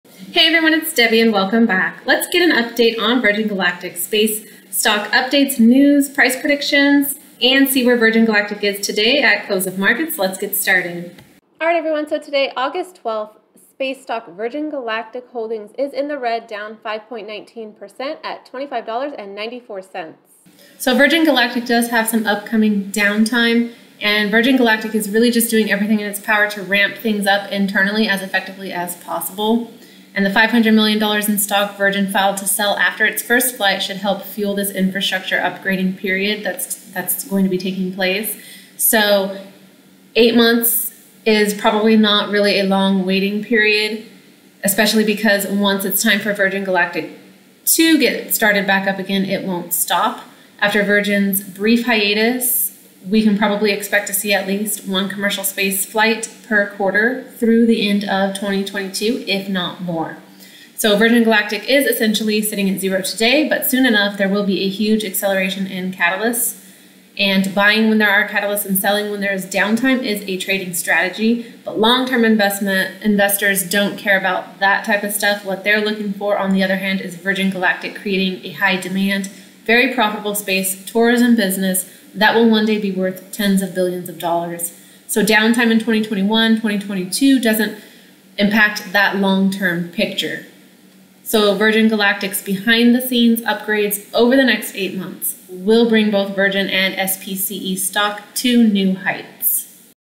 SPCE Stock Virgin Galactic Predictions for Today
SPCE stock Virgin Galactic price prediction, analysis, forecast, news updates for today.

Virgin Galactic launch
test flight
live
stock moe